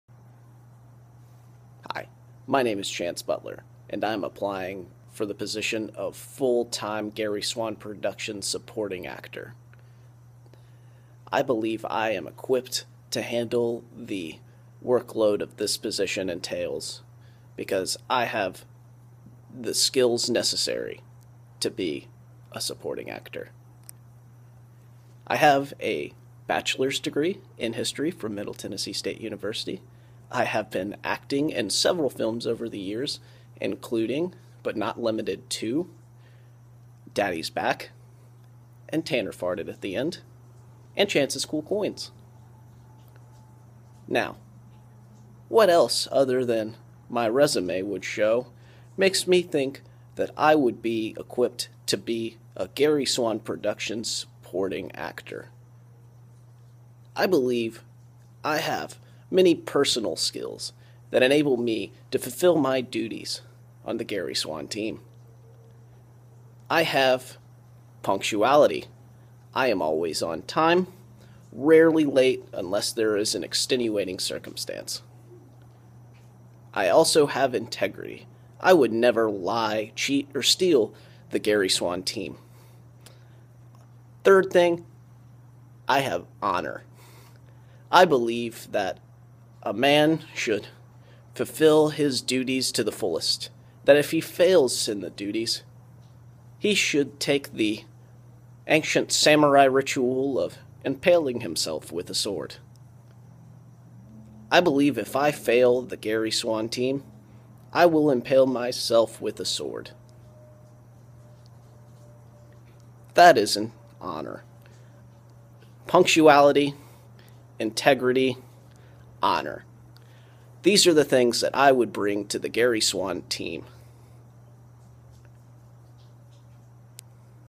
Application for Full Time Supporting Actor
Chance Butler Applies for the position of Full time Supporting actor for the Gary Swann production team.